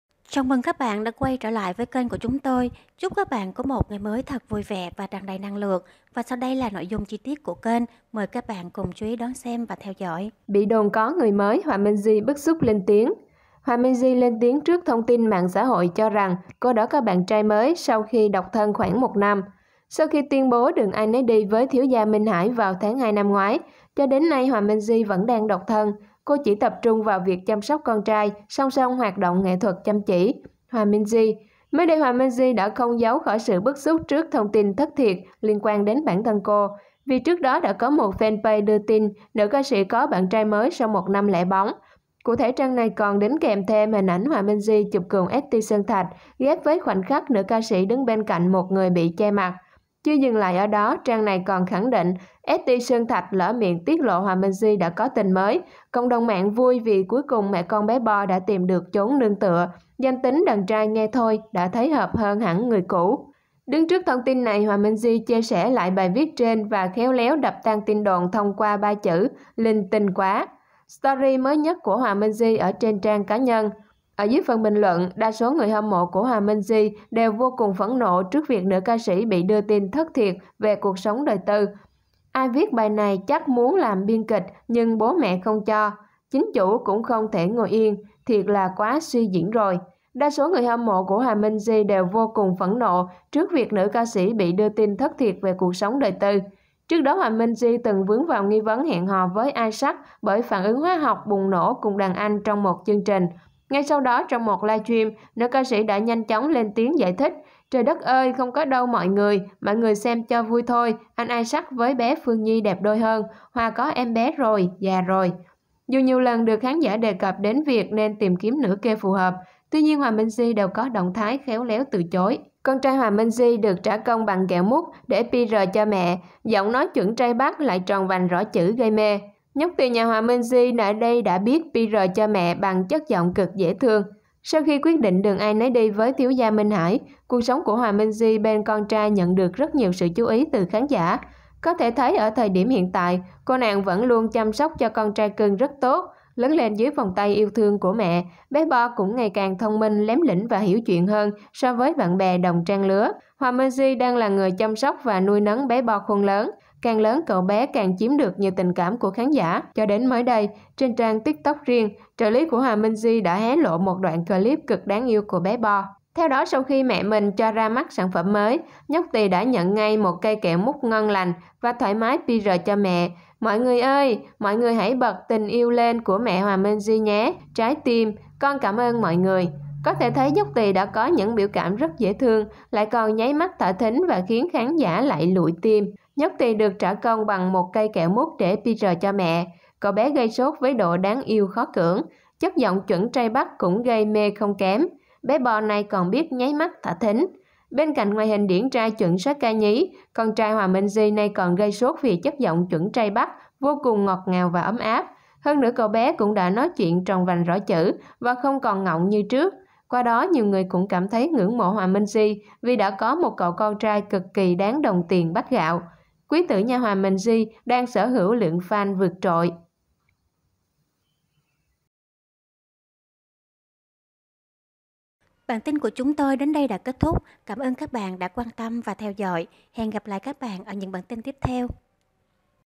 Bị đồn có ‘Bạn trai mới’, Hòa Minzy làm "lớn chuyện" bức xúc lên tiếng đáp trả
Bị đồn có ‘người mới’, Hòa Minzy  làm "lớn chuyện" bức xúc lên tiếng đáp trả